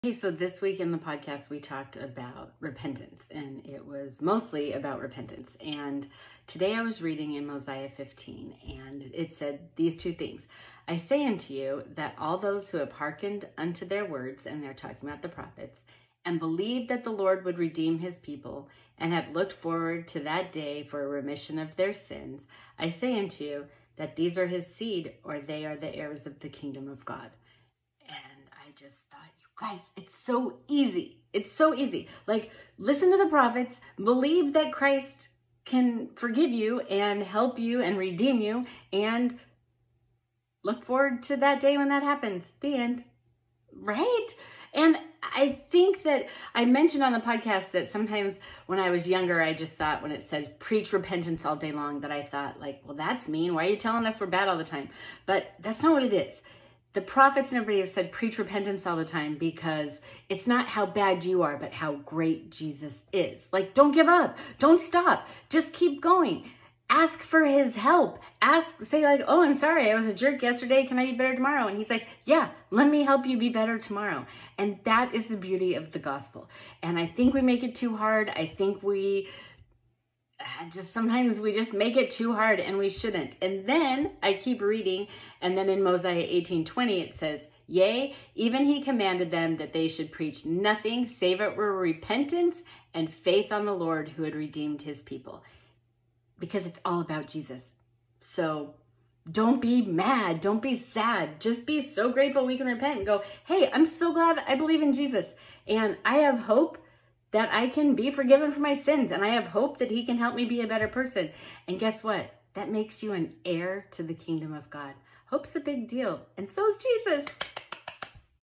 Repentance All Day Long
Give yourself a break.  It’s easier than you think.  Just repent and try harder tomorrow! We are so lucky to have a Savior.  We are so lucky to have Jesus.  Of course there are some ordinances we need - like baptism and the gift if the Holy Ghost.  But when it comes down to it, we just need to be more grateful for repentance and more thankful for Jesus.  He’s so great!! 👏🏻👏🏻👏🏻❤️❤️❤️
Haha - I didn’t say, “EAT better tomorrow.” I said, “BE better tomorrow.” Although, for me it’s probably both… 😂😂